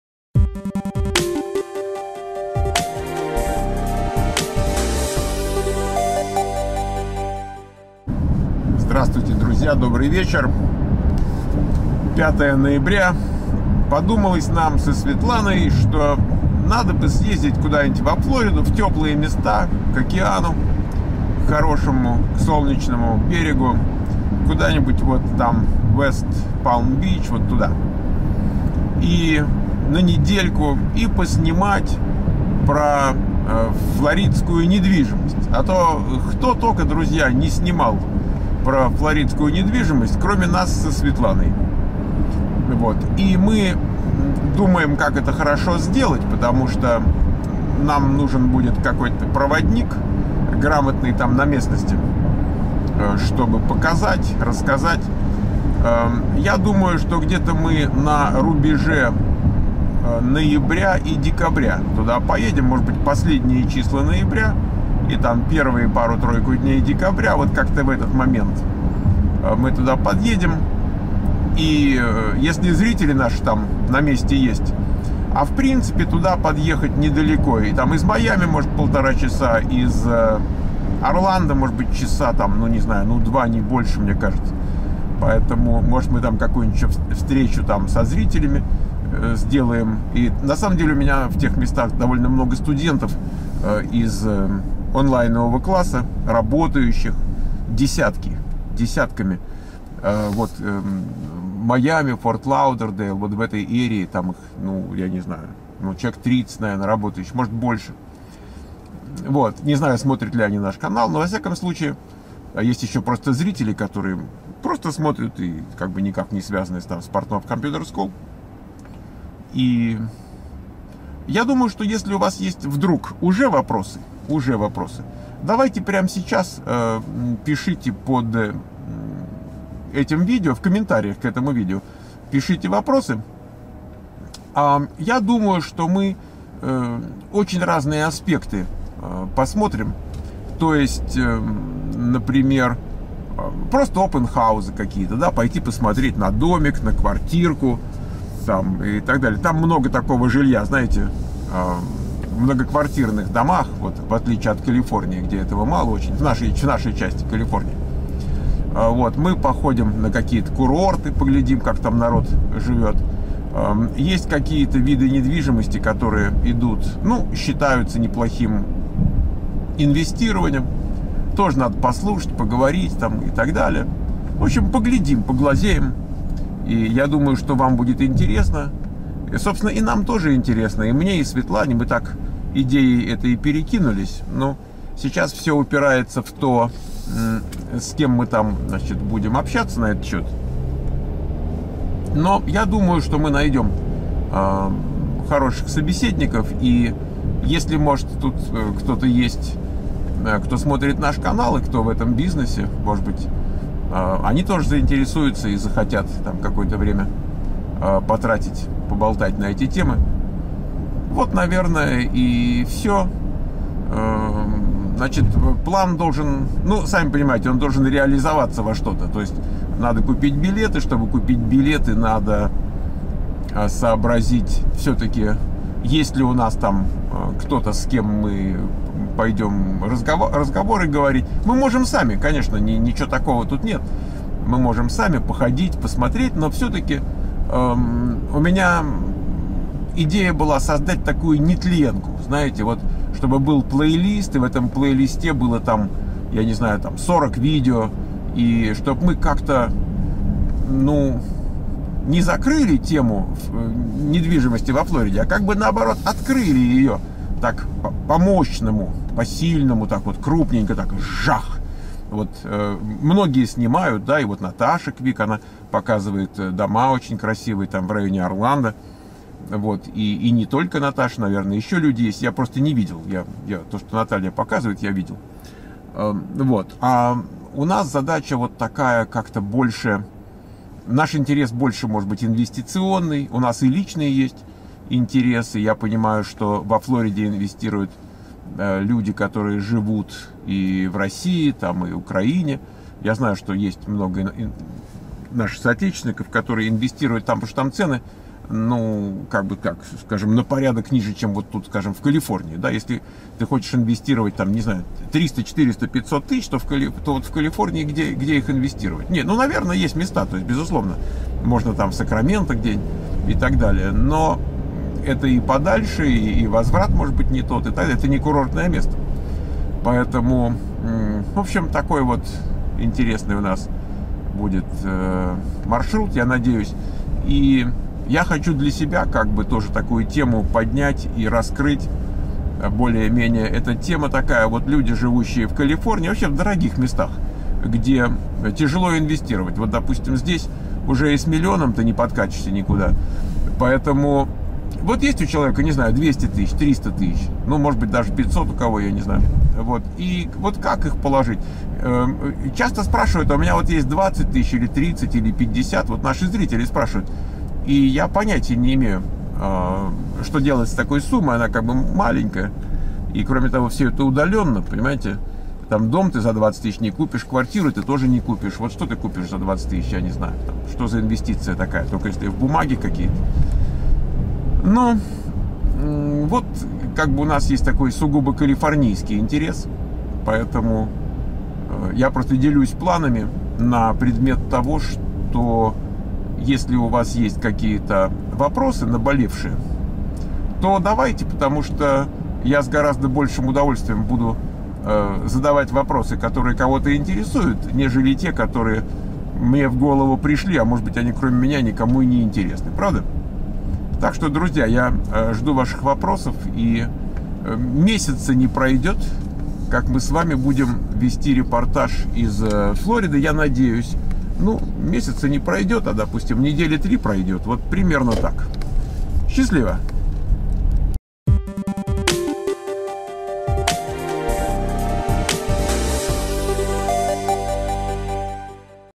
Во Флориду на недельку поехать? Неплохая идея!!!
Поедем со Светланой снимать сериал про флоридскую недвижимость
Ссылки на канале SiliconValleyVoice /школа Михаил Портнов: